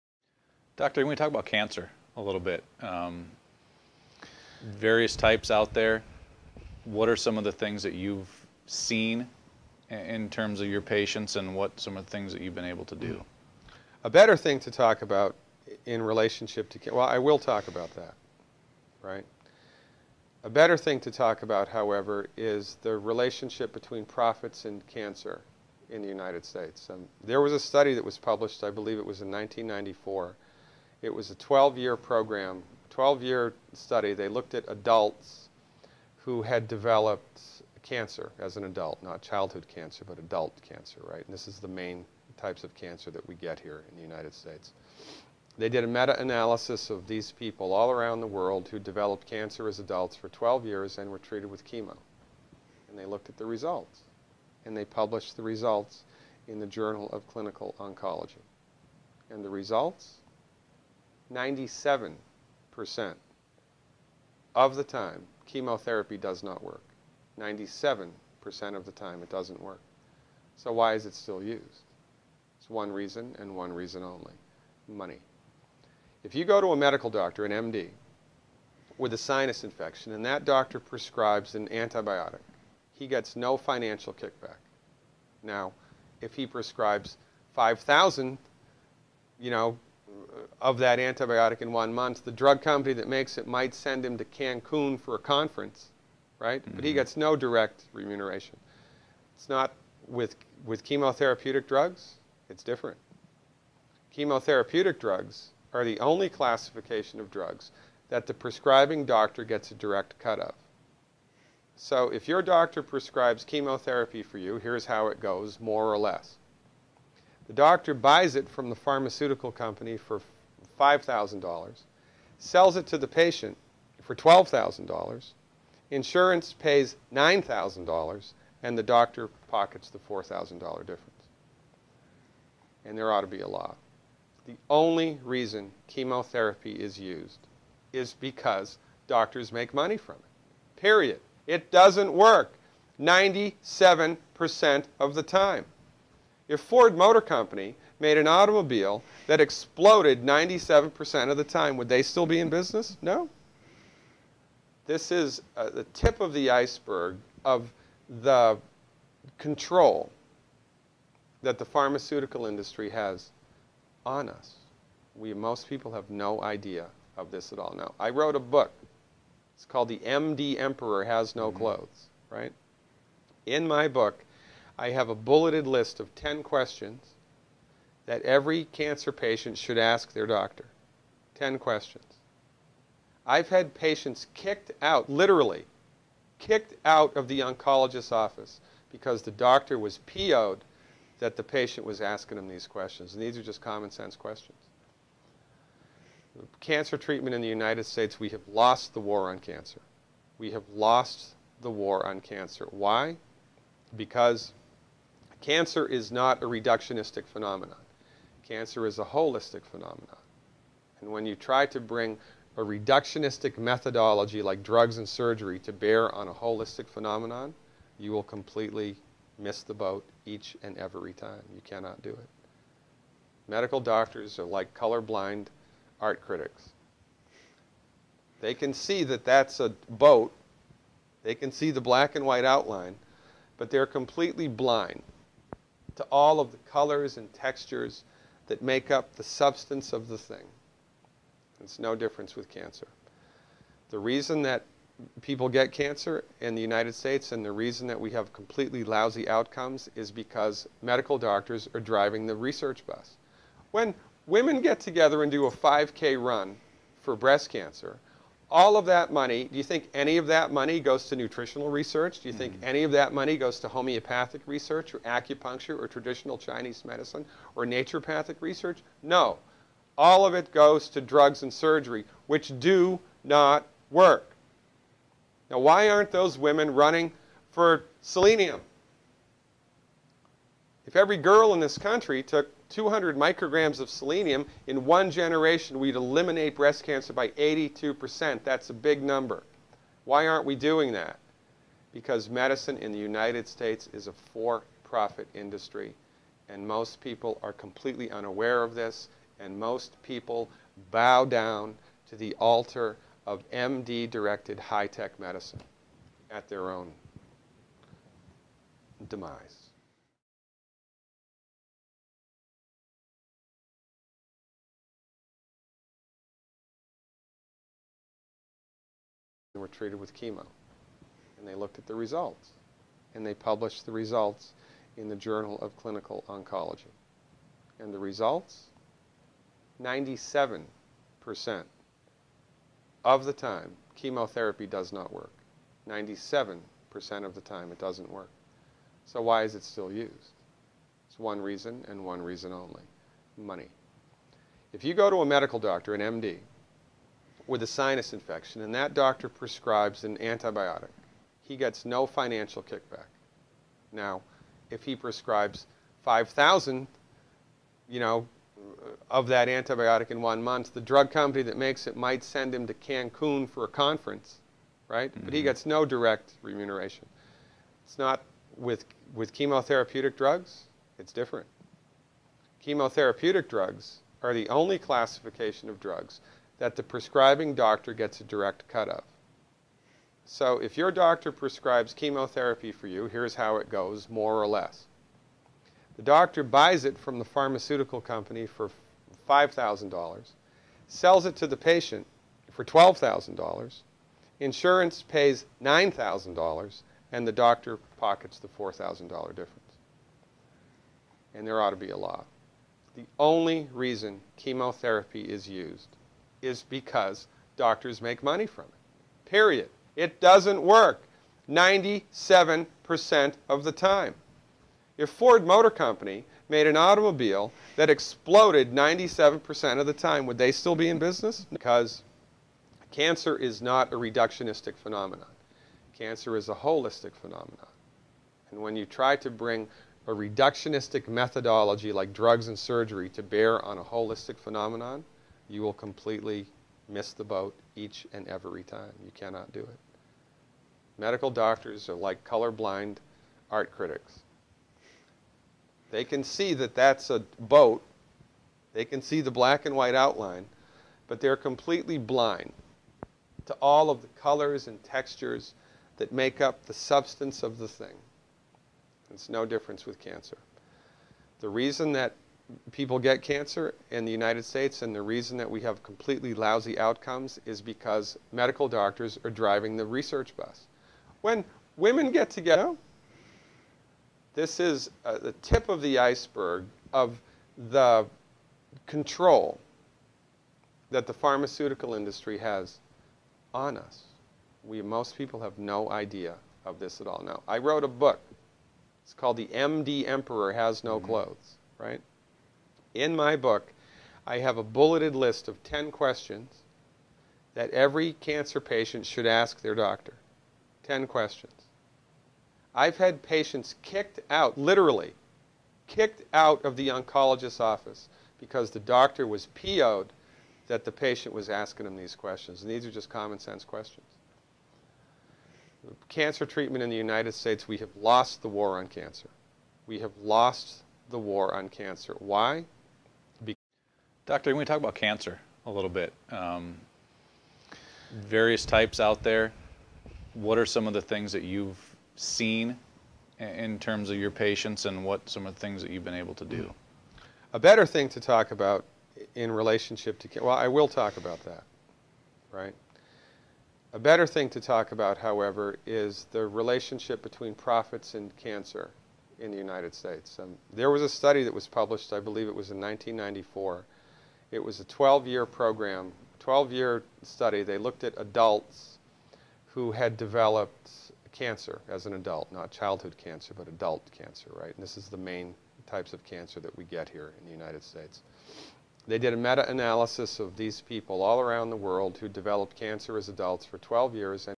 Dr. Glidden: Chemotherapy does NOT Work 97% of the time!
Chemotherapy does NOT Work 97% of the time AND kills 97% of the times! Dr. Peter Glidden (DRGLIDDEN.COM) explains why we still have a 'war on cancer'.